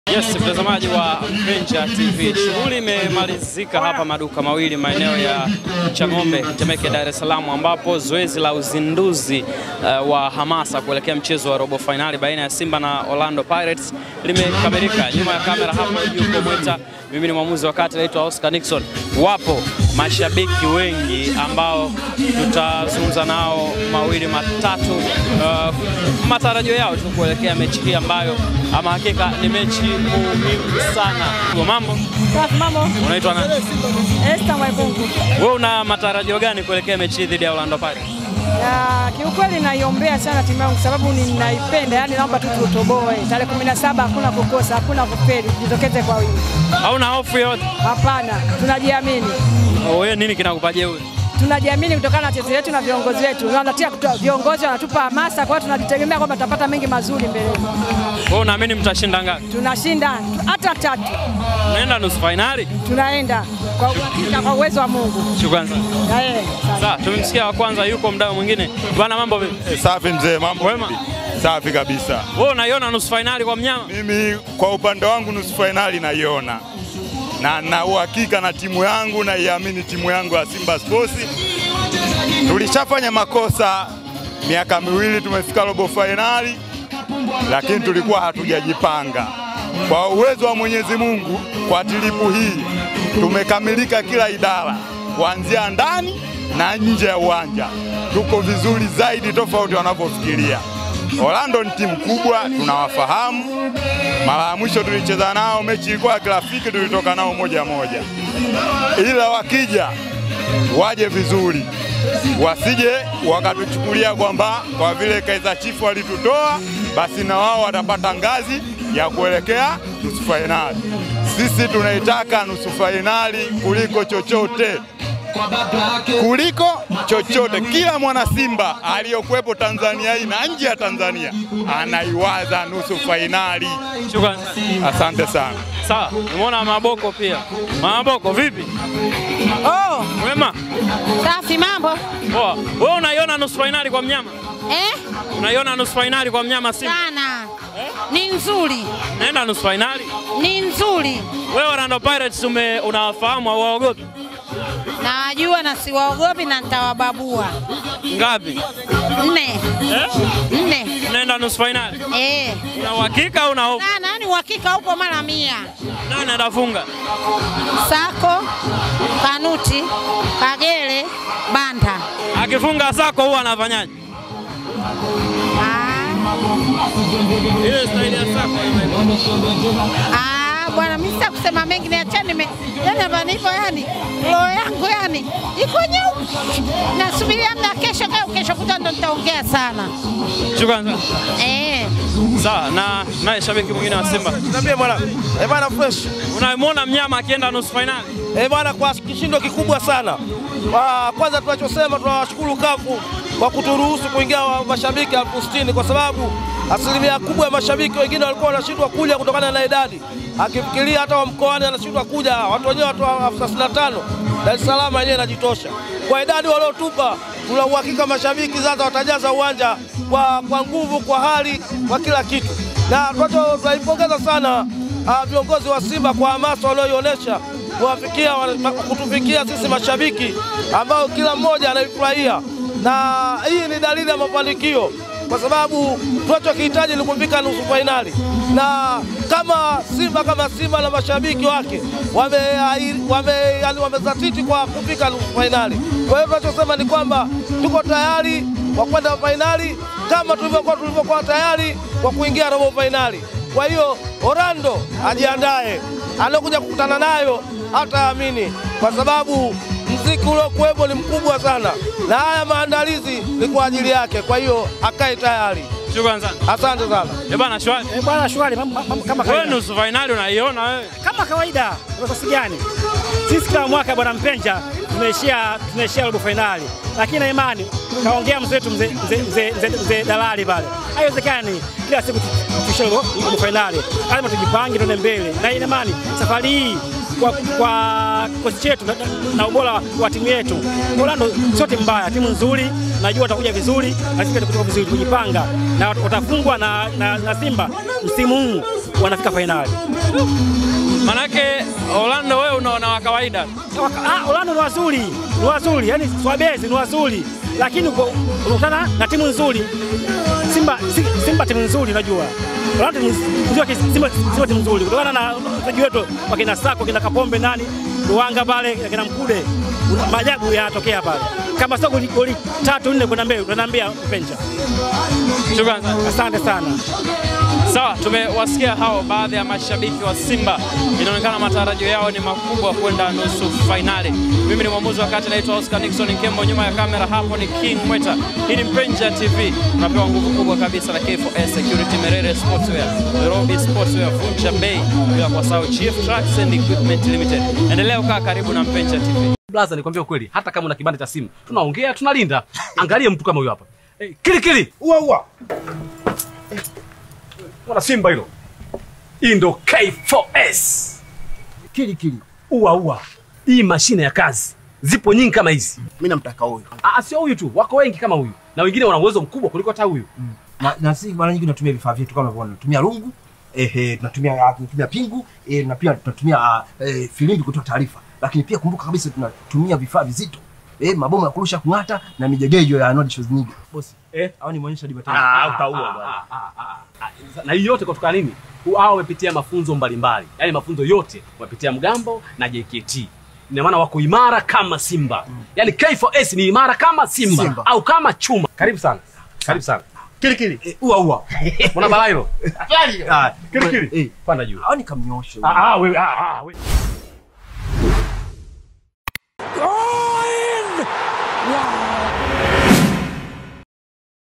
SIMBA 'TUNAANZA UPYA' /MNYAMA ANAKWENDA NUSU FAINALI/MASHABIKI WATABIRI KWA MKAPA/SIKIA TAMBO ZAO
Leo Jumnne April 12,2022 Meneja Habari na Mawasiliano wa Simba SC Ahmed Ally kuzindua kampeni maalumu ya hamasa kuelekea mchezo wa robo fainali ya Kombe la shirikisho barani Afrika ambapo Simba Itaminyana na Orlando Pirates ya Afrika Kusini na mchezo wa kwanza utapigwa Uwanja wa Benjamin Mkapa Dar es Saalam April 17 Saa 19:00 Usiku huku Shirikisho la Soka Barani Afrika CAF likiwa limeruhusu mashabiki elfu Sitini kushuhudia mchezo huo

Mpenja TV Tumefika Eneo la maduka mawili Chang’ombe Temeke Dar es Salaam ambapo Shughuli ya Uzinduzi wa Hamasa Inazinduliwa rasmi na Tunakuletea kila nukta ya jambo